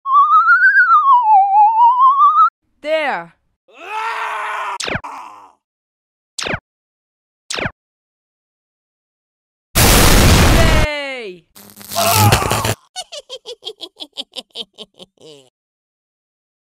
when_aliens_attack.mpg
An animation by youths at BoxHill community Arts Centre.